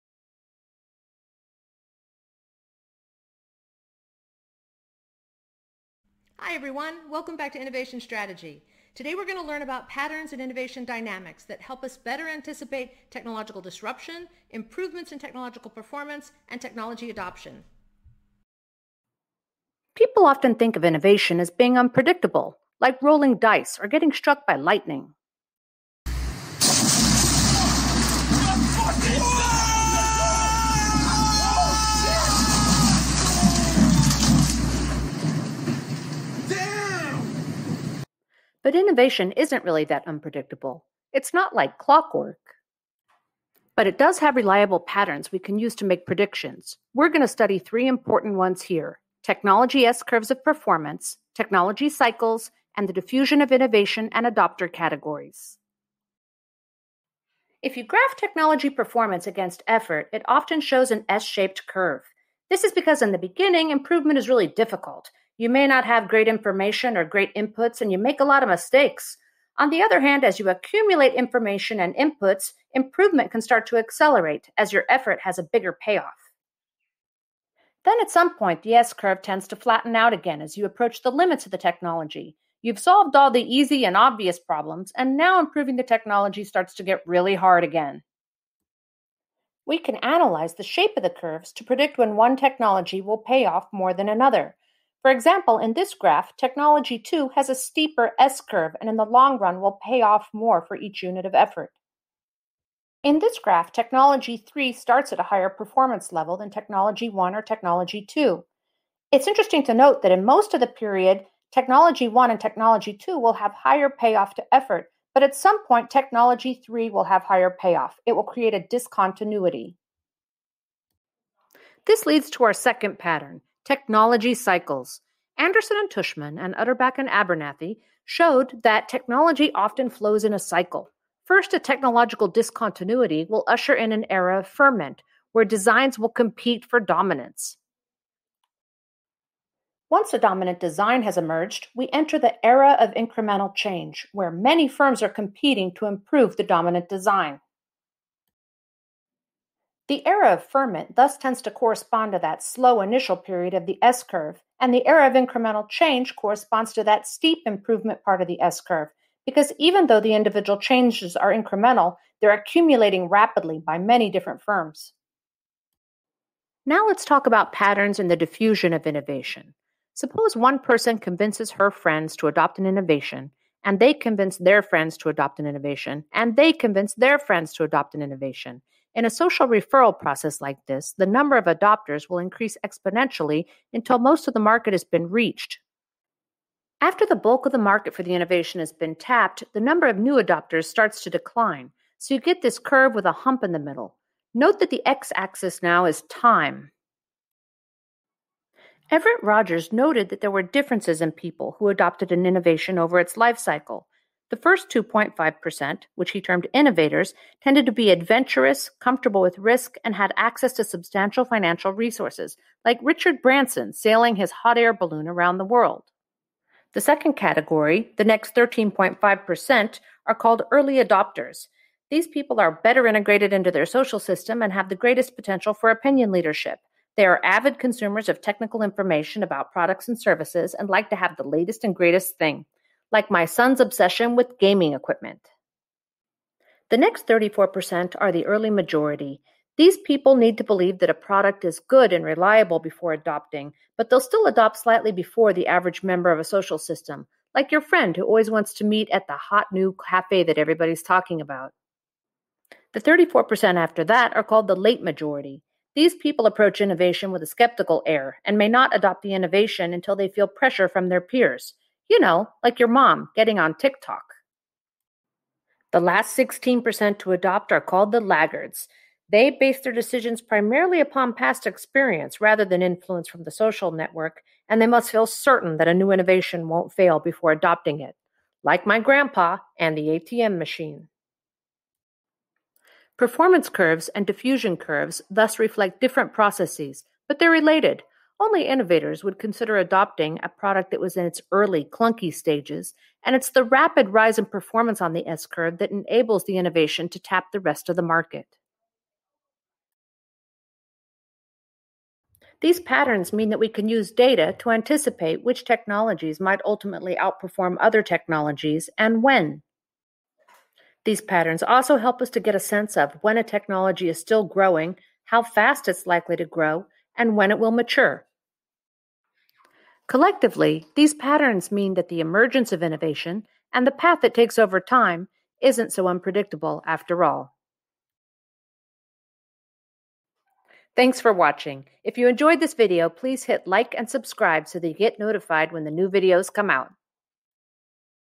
Innovation Strategy: Patterns of Innovation
This video explains patterns of innovation dynamics that can help us better anticipate technology disruption, improvements in technology performance, and innovation adoption. It covers s-curves in technology performance, technology cycles, and diffusion of innovation.